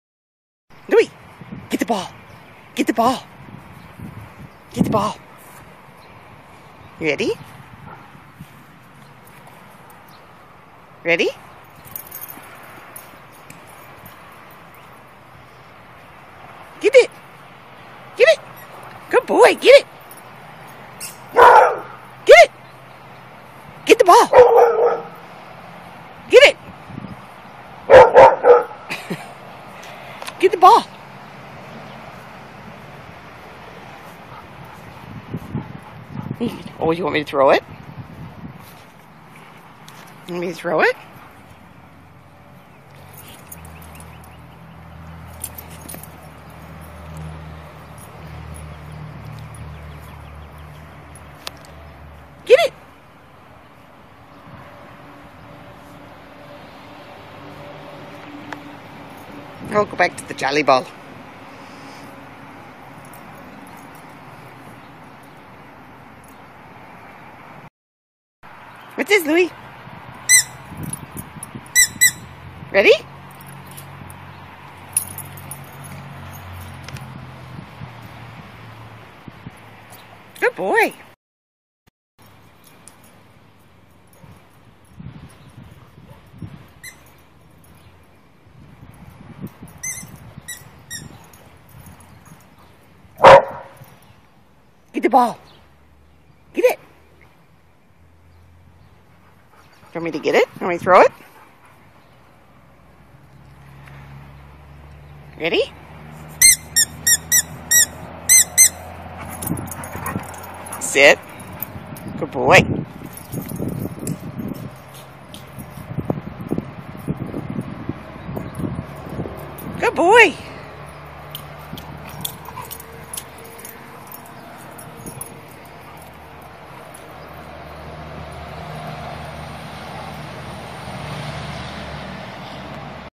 Louie with Lula's squeaky ball
Louie discovered Lula's squeaky ball this morning. At first, all he did was bark at it and bounce around it. After a few tosses, he figured out how to pick it up and that the squeaking was kind of fun! Pretty good for a sound sensitive dog! Love seeing progress!!!